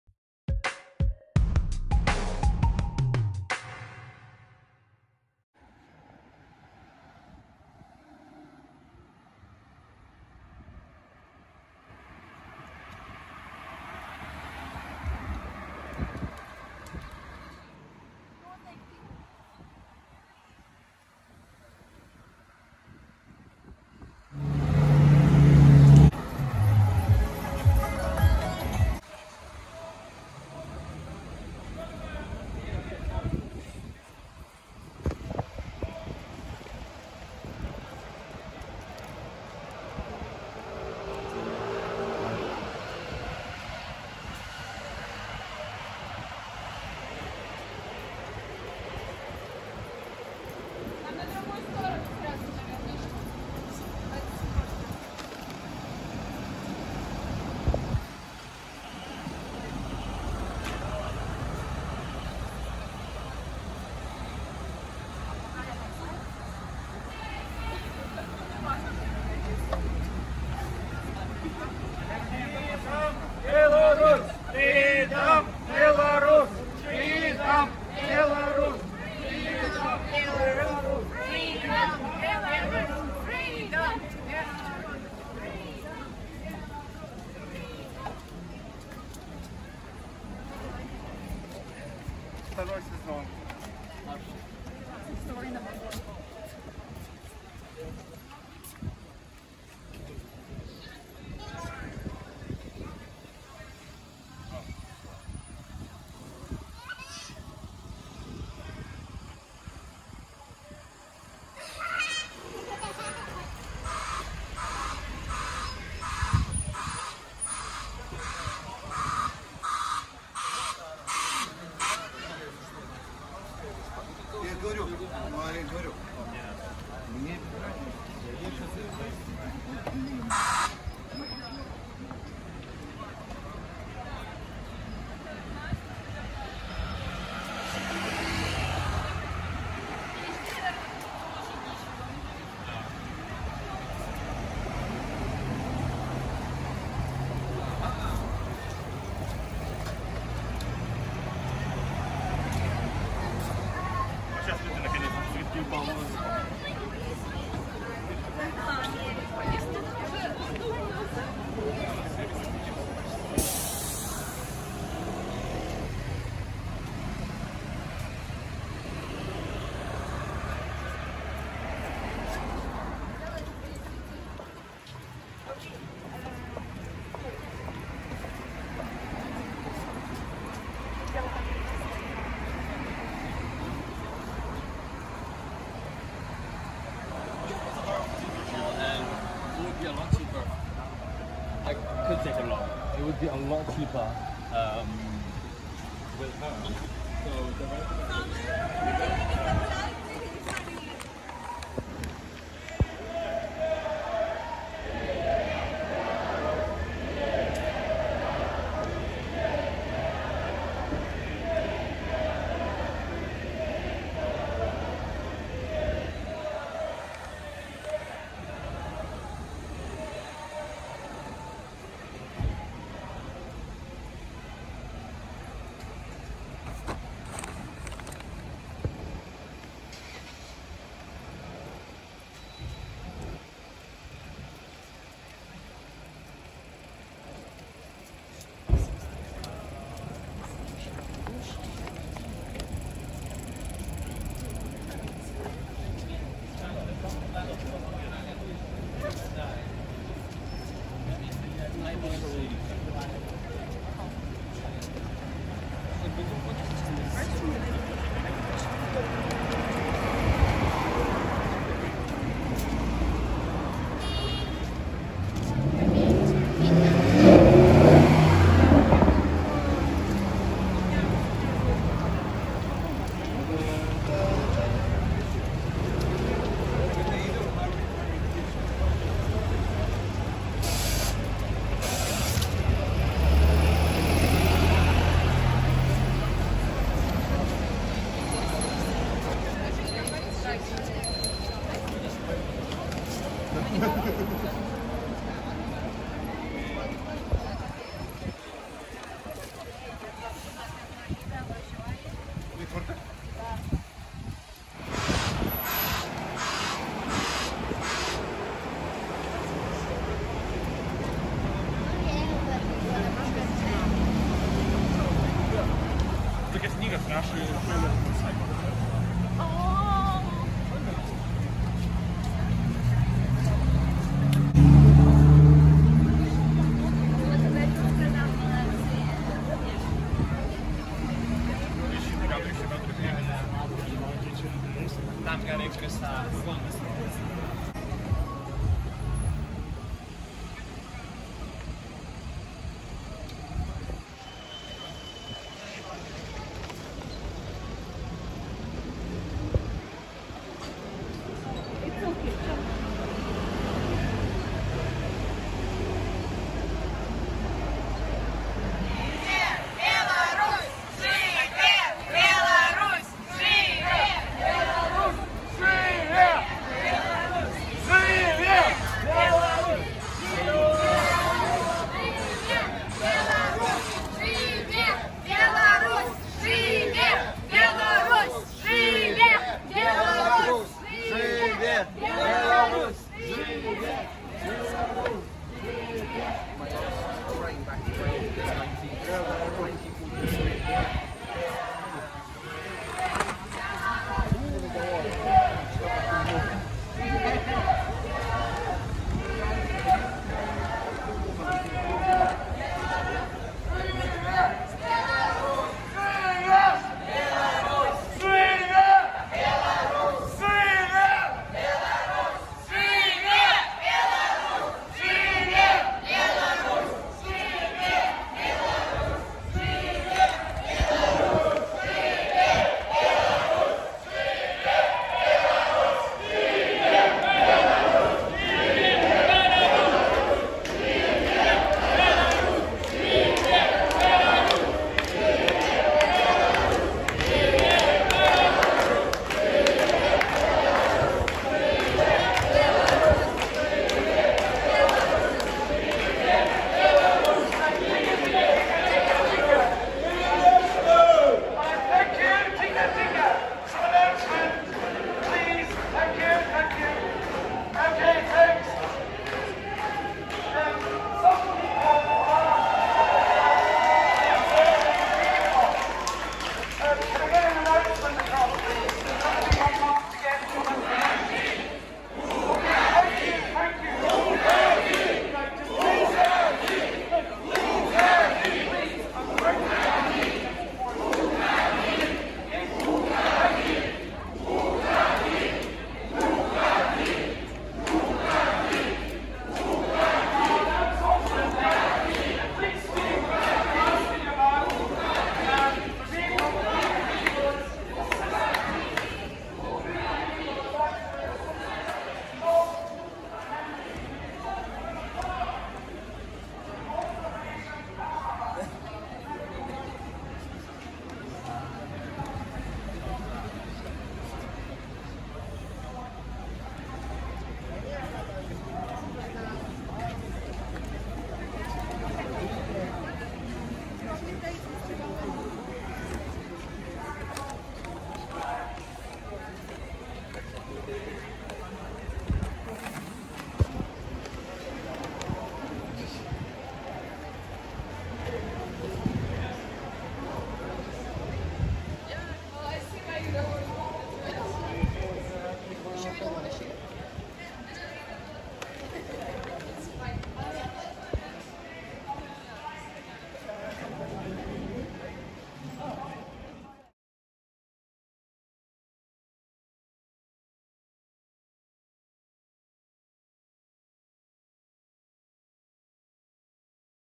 CAR SPOTTING Interrupted By BELARUS Protestors In LONDON | Demand Corrupt LUKASHENKO To Resign |4K
Car Spotting Interrupted By BELARUS Protesters | Demand Corrupt LUKASHENKO To Resign